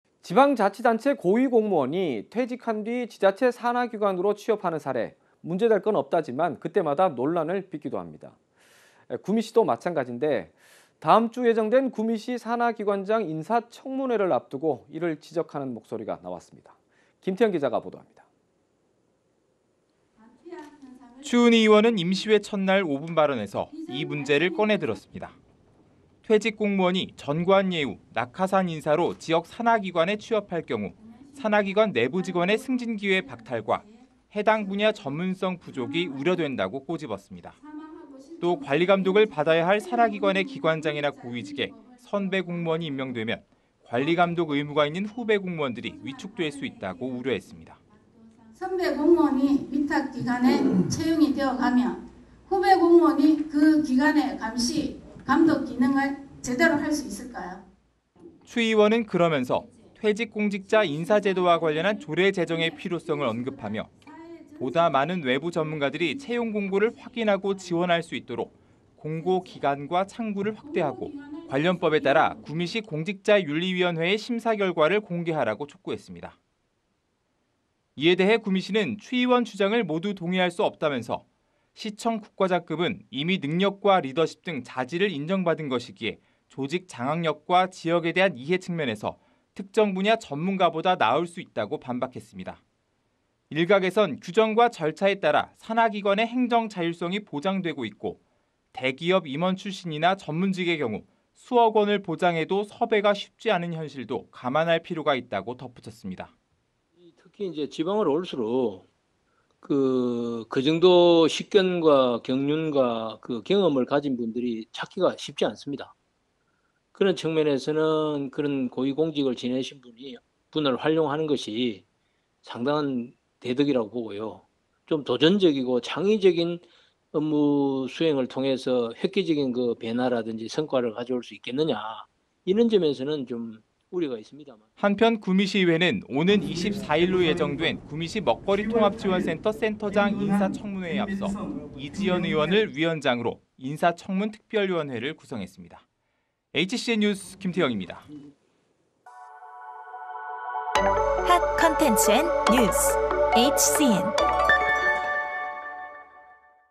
퇴직 공무원의 산하기관 영입…문제 없나/HCN새로넷방송
[앵커멘트]

지방자치단체 고위 공무원이 퇴직한 뒤
지자체 산하기관으로 취업하는 사례,
문제될 건 없다지만,
그 때마다 논란을 빚기도 합니다.

구미시도 마찬가진데,
다음 주 예정된 구미시 산하기관장
인사청문회를 앞두고 이를 지적하는 목소리가
나왔습니다.

김태형 기자가 보도합니다.